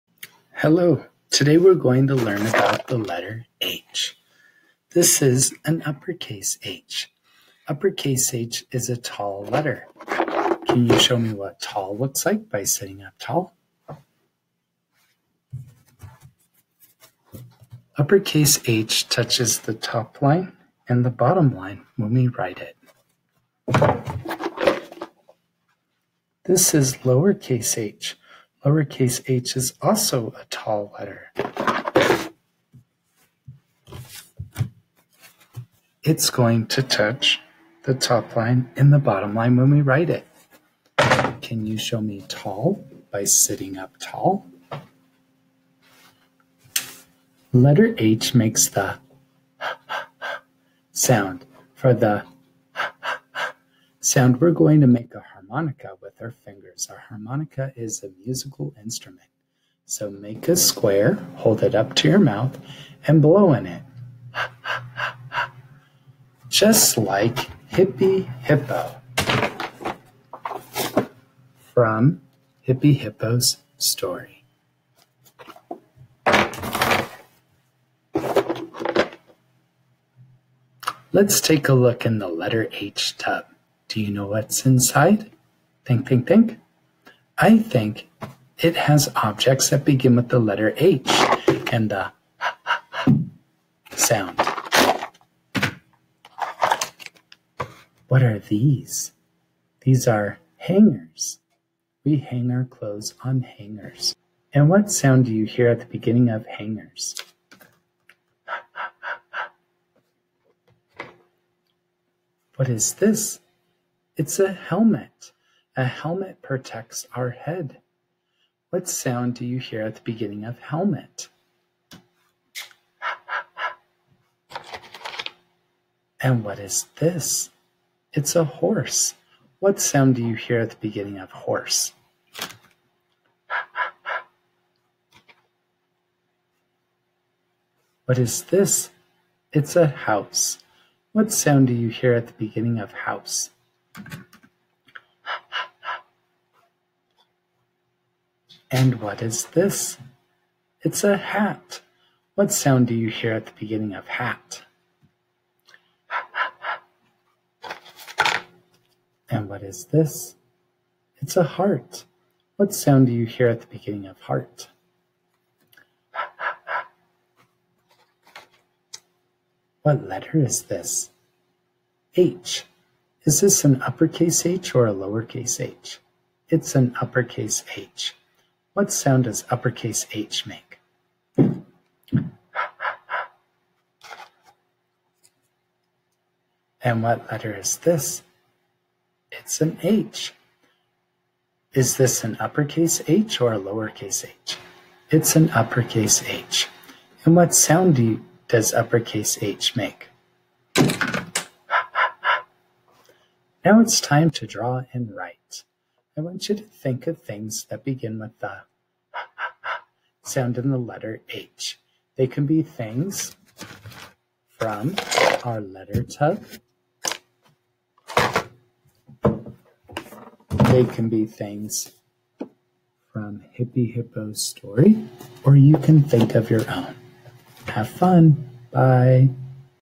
Alphabet Tub Hh
Curtis Ferlisi (an elementary teacher) teaches uppercase H and lowercase h letter name and sound using the Lakeshore Sounds Teaching Tub. Children will expand their vocabulary learning objects that begin with the letter H or h. Making connections and Total Physical Response (TPR) are used to help reinforce what is learned.